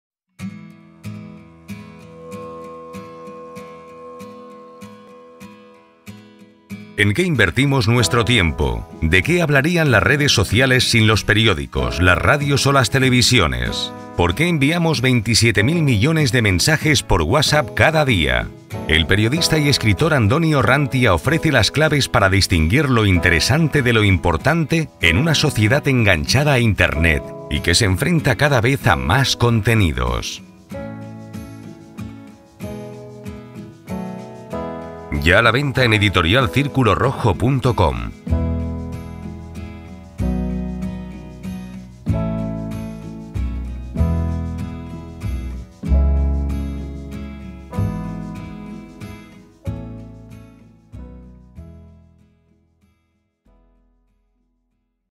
¿Qué es noticia? (Booktrailer) - Editorial Círculo Rojo
¿Por qué más de 6 millones de españoles están enganchados a Internet las 24 horas?. ¿Cómo se hubiera vivido el 23-F con Twitter? ¿Por qué se venden más iPhones que niños nacen en un día?. ¿En qué invertimos nuestro tiempo?. ¿Por qué enviamos 27.000 millones de mensajes por WhatsApp cada día?.
A lo largo de sus páginas, ¿Qué es notici@? analiza el comportamiento de una sociedad pantallizada en la que gracias a Internet el acceso a la información se ha generalizado. Donde Twitter y WhatsApp han sustituido al tradicional café. Donde necesitamos guías para distinguir lo interesante y lo importante de ese contenido que sigue siendo el mismo que hace años pero cuya forma de presentarlo ha cambiado.